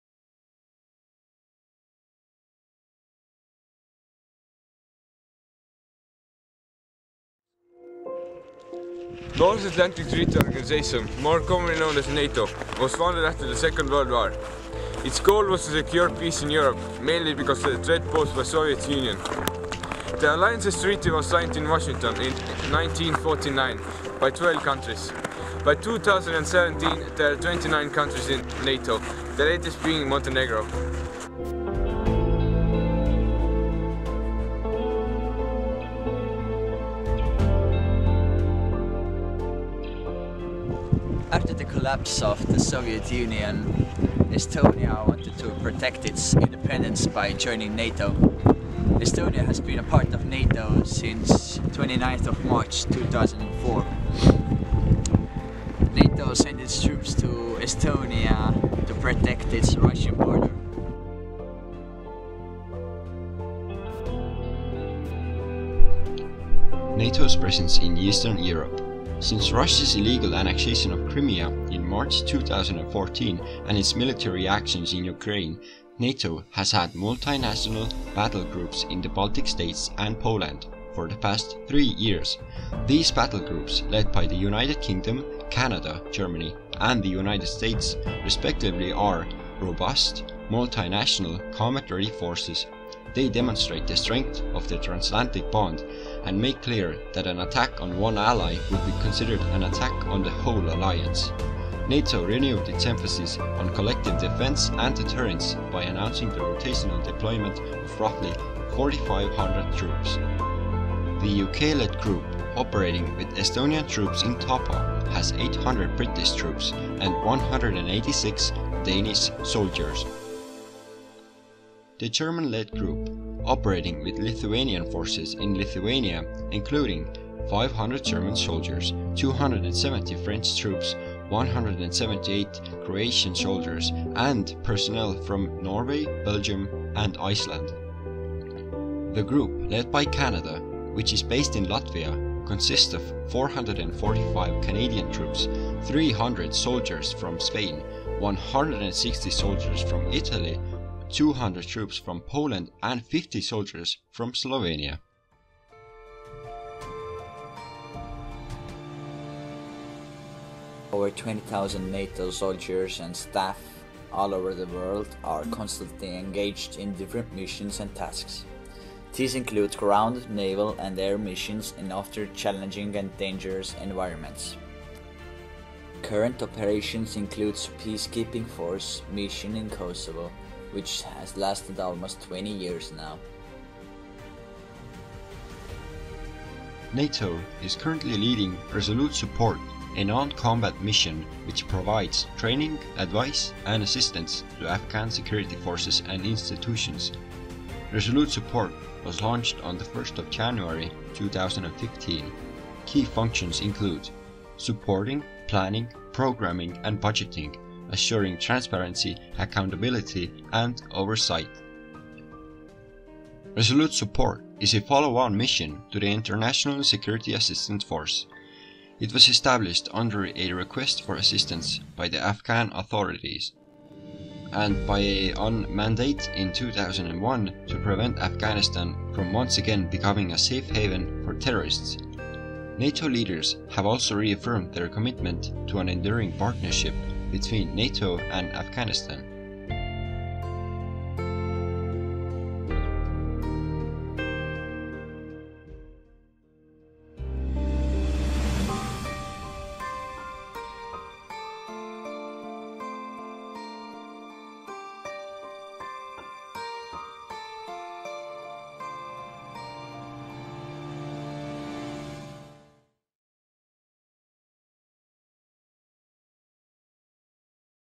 Eesti NATO Ühingu noorte video loomekonkursi IV koht.
Video pealkiri: NATO70Eesti15 loomekonkurss   N70Estonia15.

Video autorid: Henri Regor Välima, Karl Oskar Palo , Marcus Aarma ja Siim Sarapu.

Juhendaja : Kristjan Kruus.

Kool: Pelgulinna Gümnaasium.